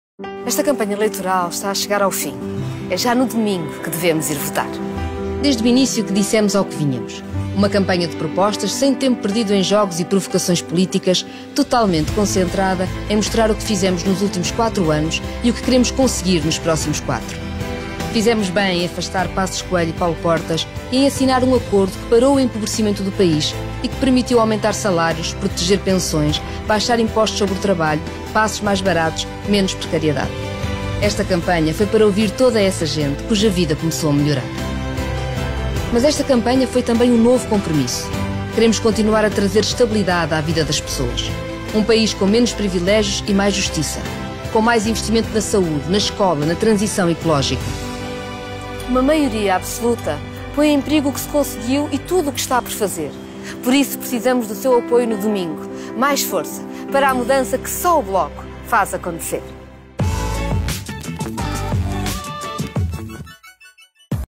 Mais força para a mudança que só o Bloco FazAcontecer | ESQUERDA.NET
Esta campanha foi para ouvir todas as pessoas cuja vida melhorou nos últimos quatro anos. Mas foi também um novo compromisso, para continuar a trazer estabilidade, menos privilégios, mais justiça, social e climática.